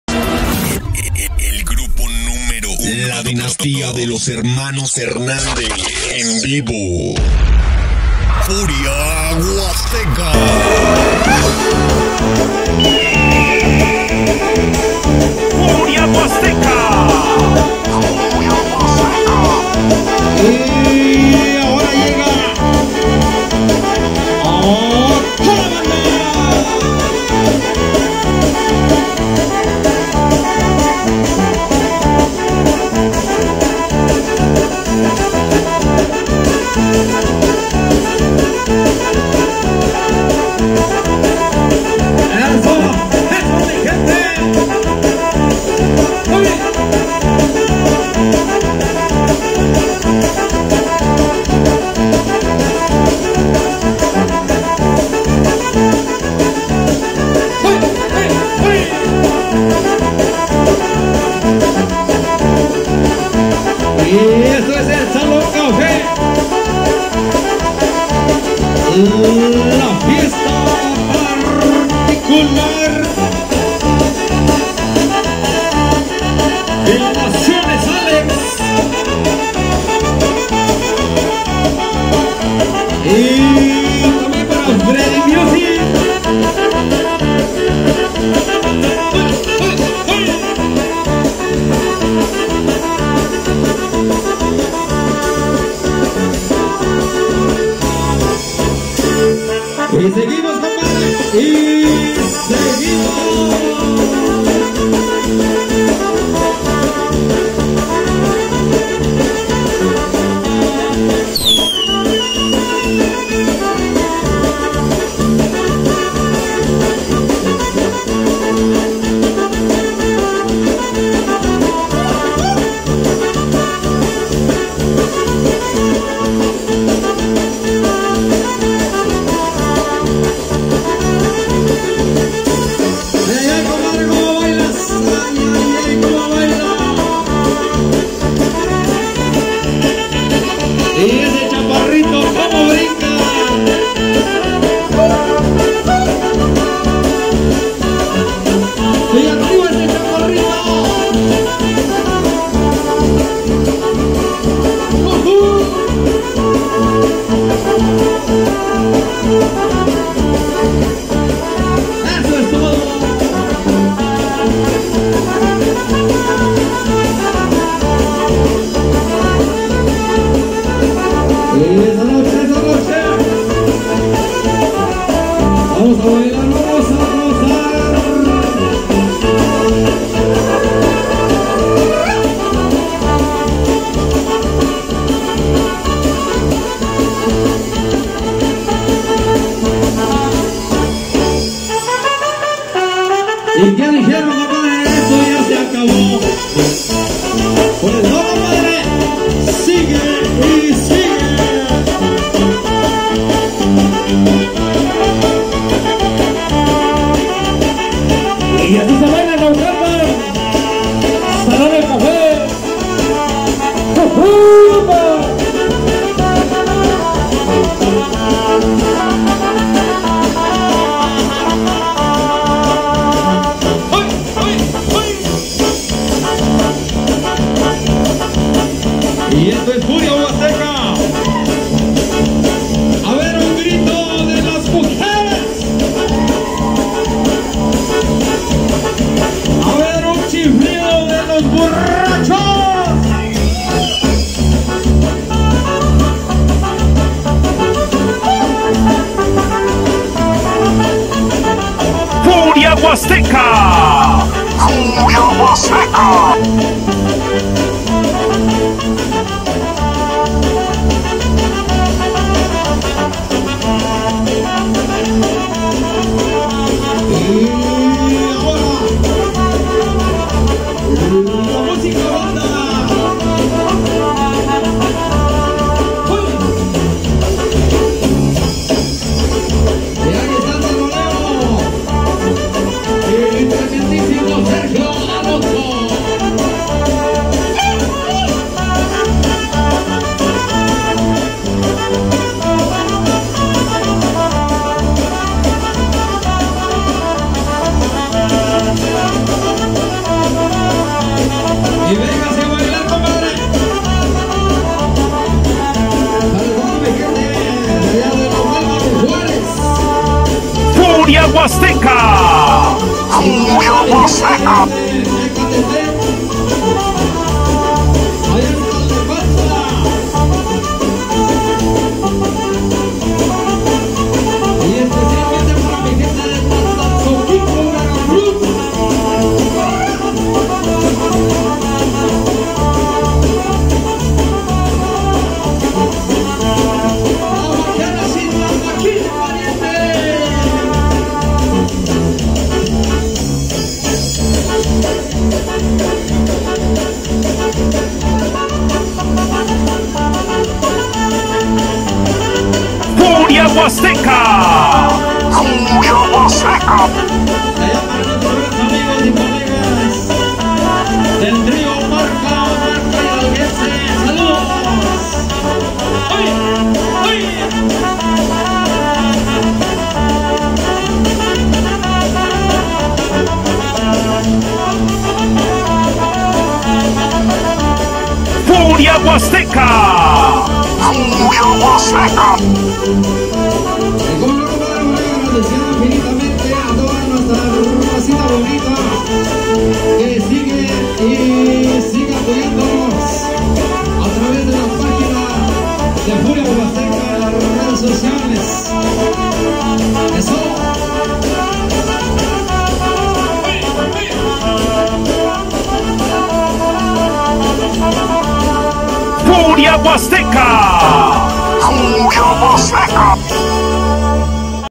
Grupo furia huasteca Dinastia Hnos Hernandez EnVivo 2023 desde salón el café🇲🇽 Naucalpan Edo Mex🇲🇽
A ritmo de banda de viento